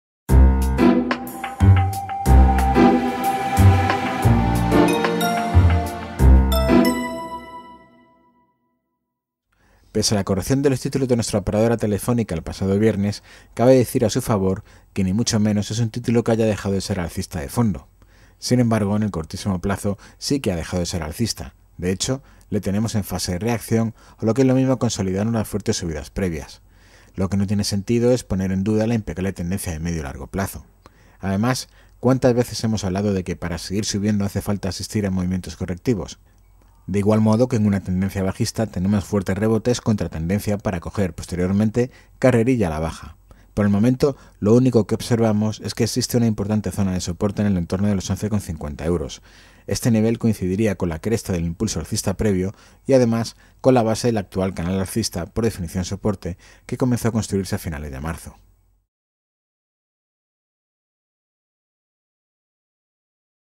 Análisis técnico de Telefónica: alcista pese a las últimas caídas
Pese a la fuerte corrección del viernes pasado, la tendencia de fondo de los títulos de Telefónica sigue siendo alcista.

Ahora bien, en el muy corto plazo no podemos descartar la vuelta a niveles de soporte cercanos a los 11,50 euros.